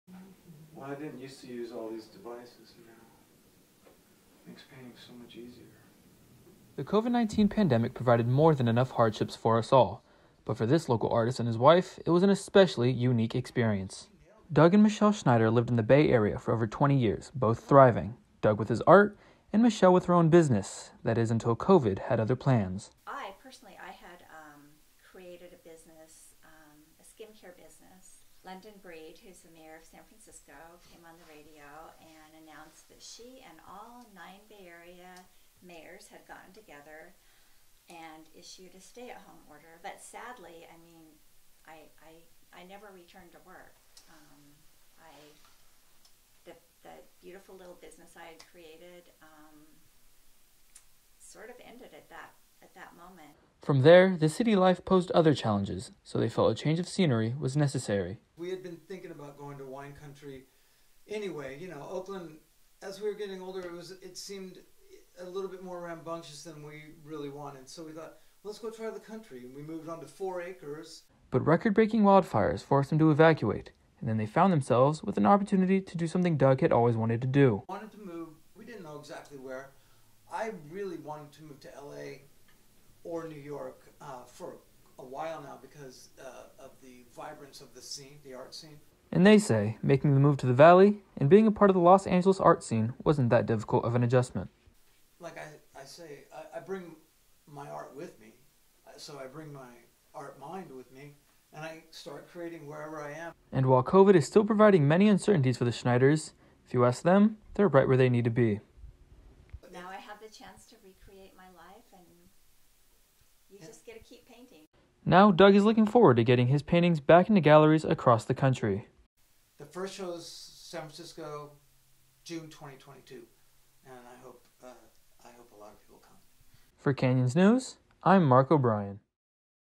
Local Artist Faces Challenges After Moving In Midst of COVID-19 Pandemic
A local artist along with his wife recently moved to the SCV from the Bay Area in pursuit of bigger and better things after the COVID-19 Pandemic provided everlasting challenges.